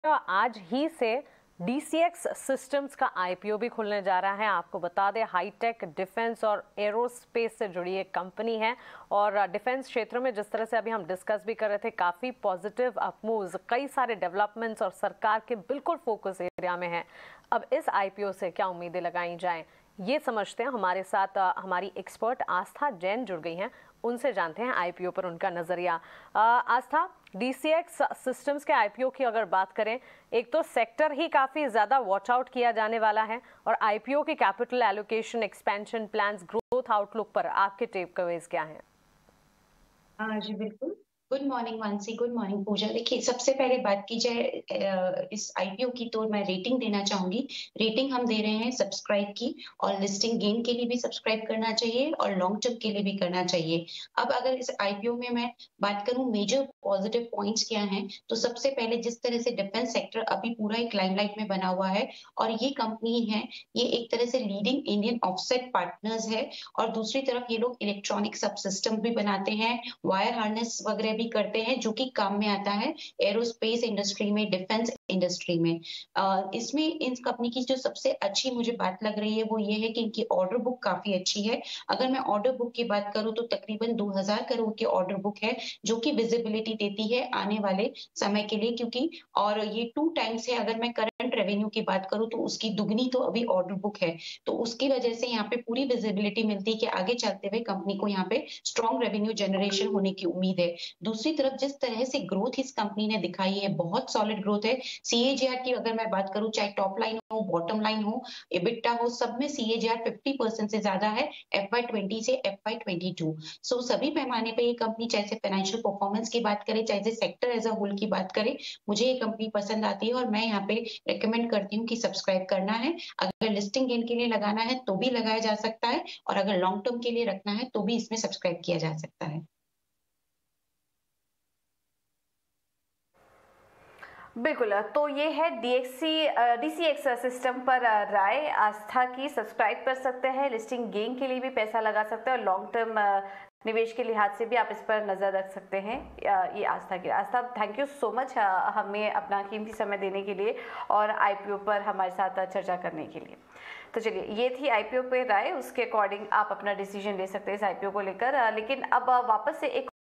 DCX System IPO को Subscribe करने पर Market Experts का क्या है नजरिया? जानें कंपनी की बारीक बातें
Share Market में जल्द होगी  Defence Sector की कंपनी DCX System की एंट्री। Primary Market में आज से खुलेगा DCX System का IPO। जानें IPO और कंपनी पर Market Expert का नजरिया।